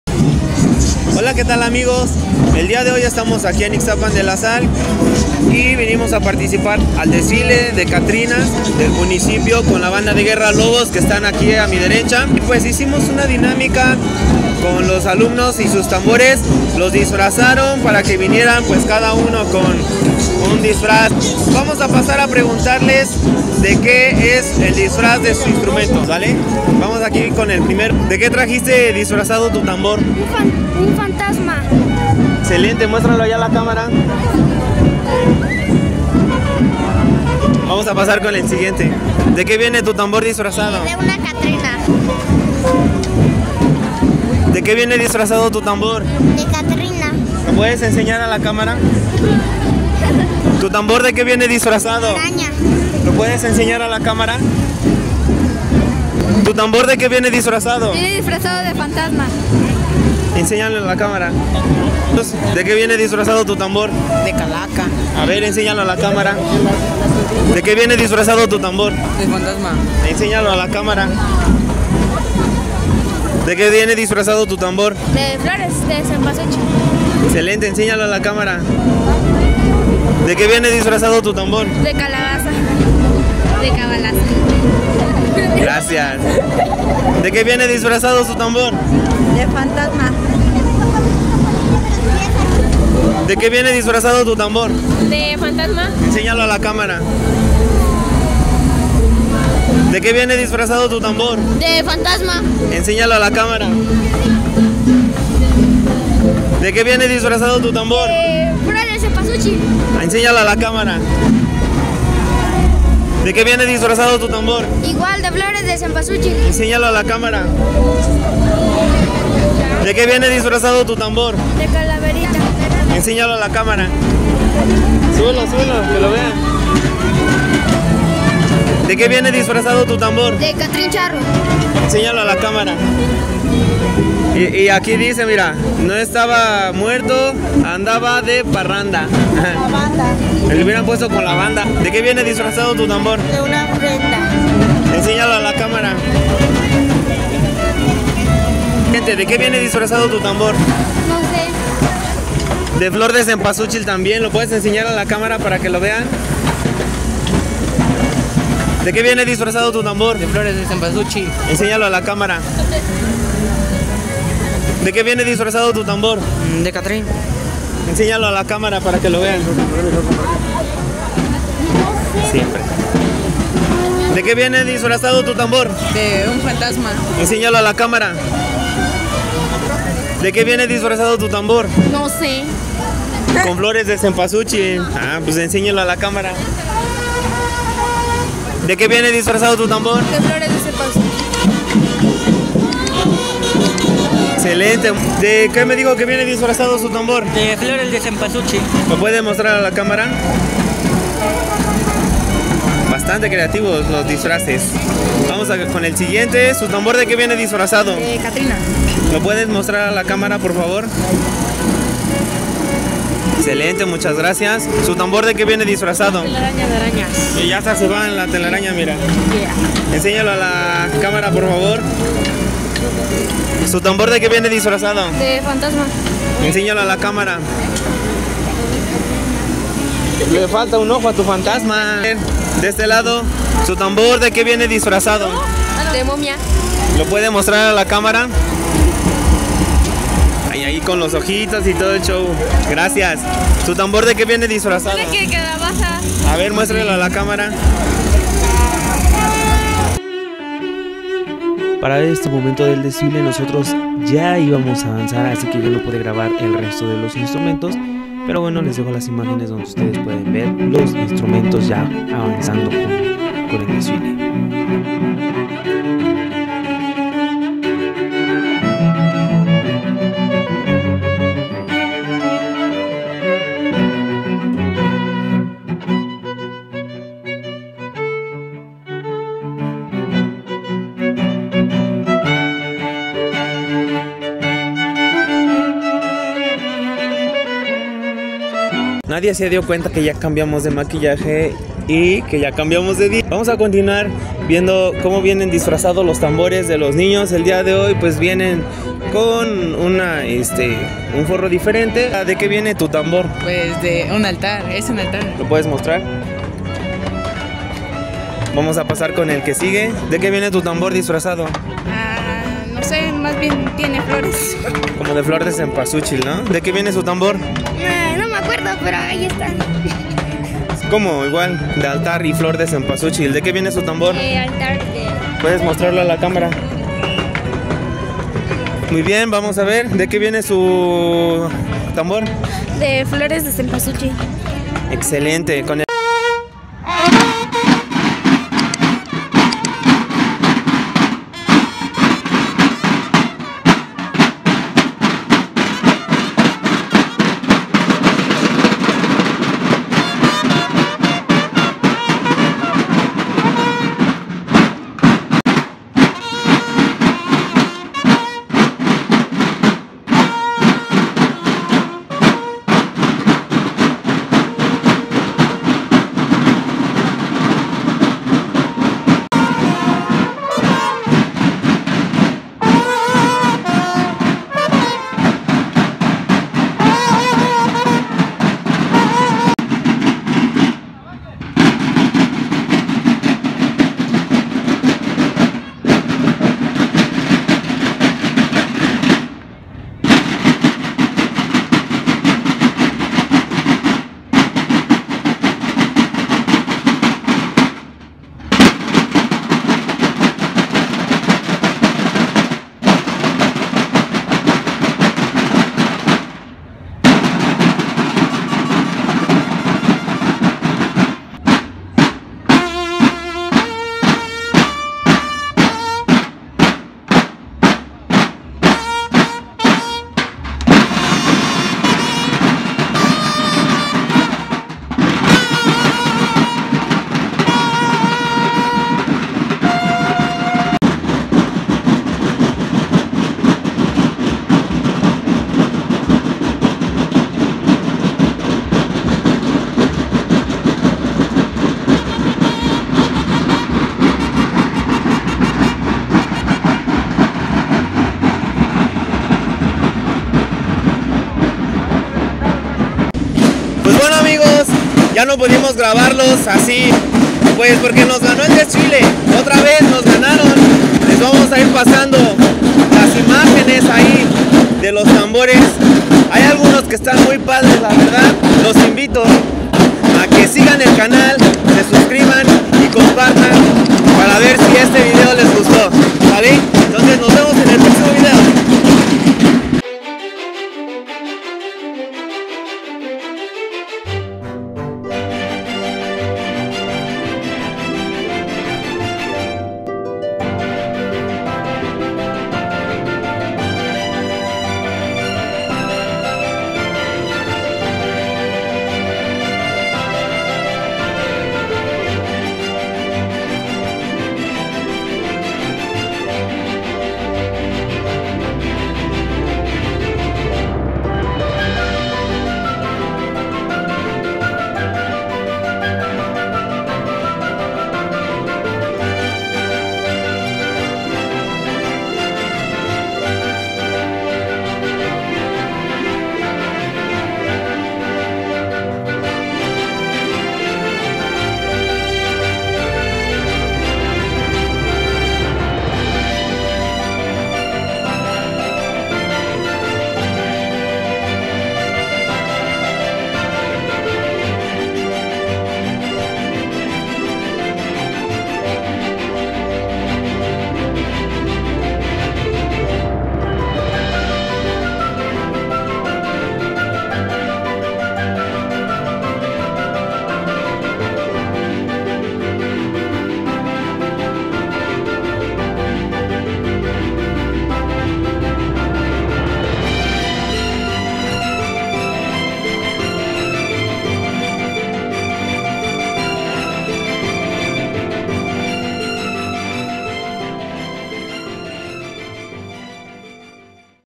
Desfile de CATRINAS en Ixtapan de la Sal con bandas de Guerra
Y si te gustaría conocer mas marchas o algún tema en especifico, "DEJAME UN COMENTARIO"